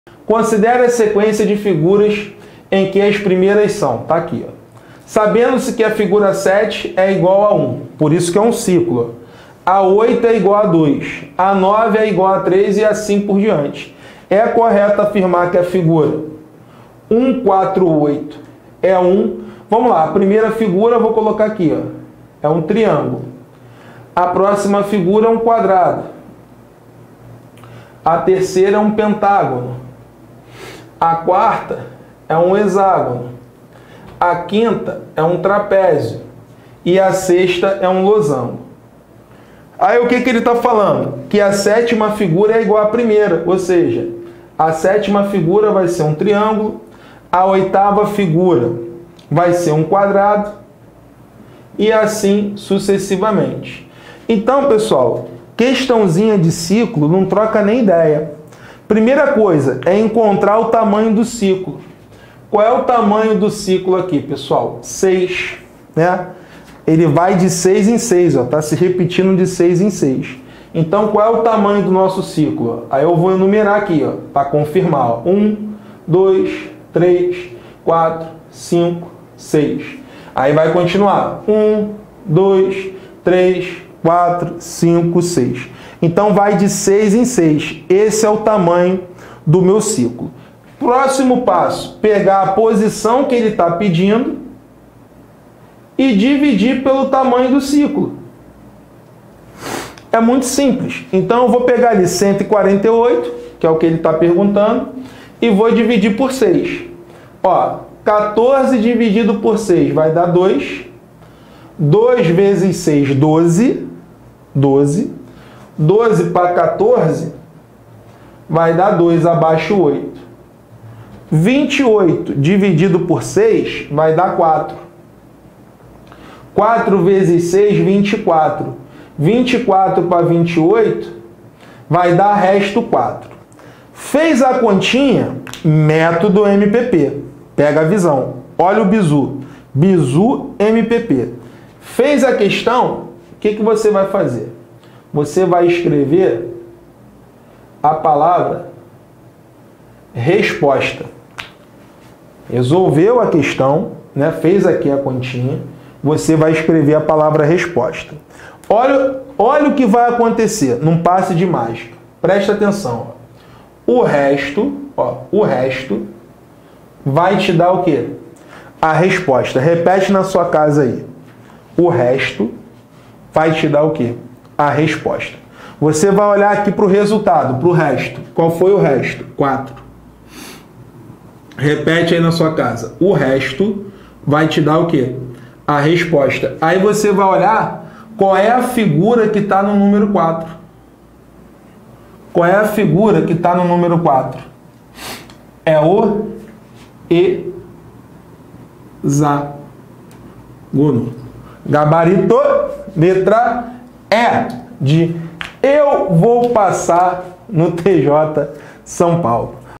Raciocínio Lógico para TJ SP |Dica 41| Sequências Lógicas Veja Como Fazer.
Você não sabe Sequências Lógicas para o TJ SP ?

Então você precisa aprender com esse vídeo onde nosso professor, Marcão, mostra Sequências Lógicas para a prova de Raciocínio Lógico do TJ SP.

Você terá um passo a passo para aprender  Sequências Lógicas para o TJ SP e nunca mais errar nenhuma questão.

Agora basta dar o play no vídeo e aprender com o Método MPP Logica de Argumentação para o TJ SP.